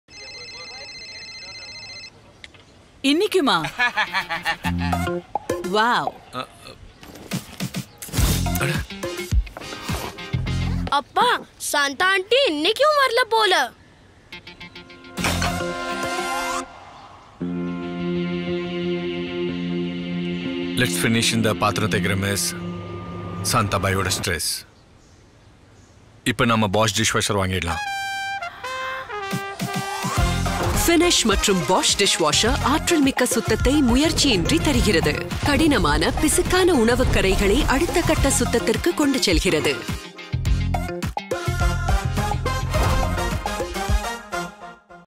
Finish Quantum x Bosch Dishwasher | Finish the stress of dishes | Tamil
Mountain of dishes to clean with no help in sight? Time to Finish the stress. Bring home a Bosch dishwasher and experience Finish Quantum tab's superior cleaning. Its 3 chamber technology fights tough stains and cuts through grease to give you the ultimate shine and clean.